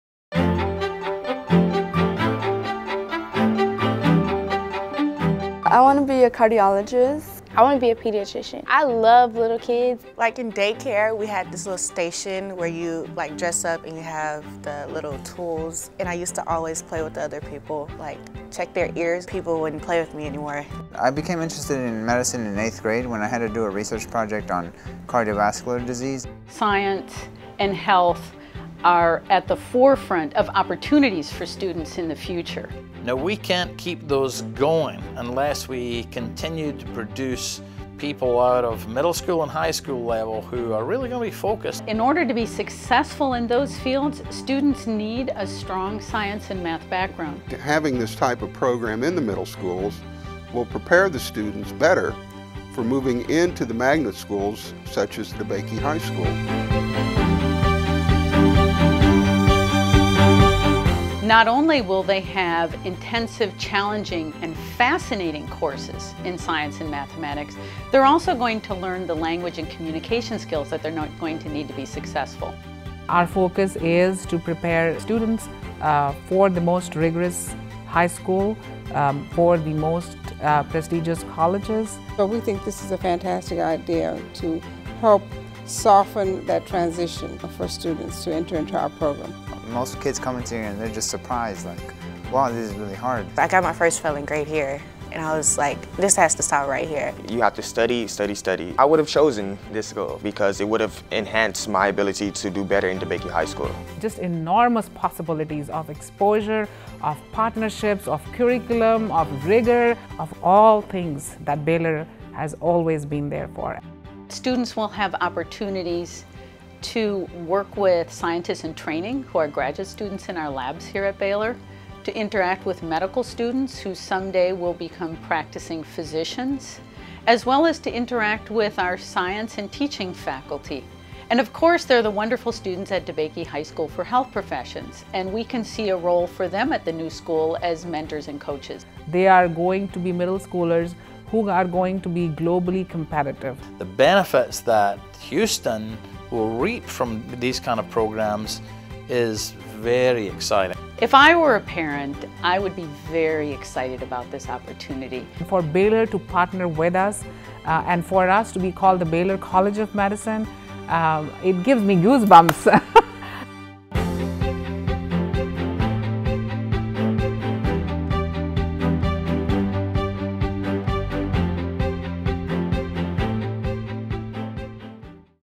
Baylor College of Medicine Middle School Academy at Ryan
Baylor College of Medicine and the Houston Independent School District are opening a new middle school that will focus on a very rigorous math and science program. The Baylor College of Medicine Academy at Ryan will expose sixth graders to physicians, scientists, students from DeBakey High School and to the fascinating world of medicine.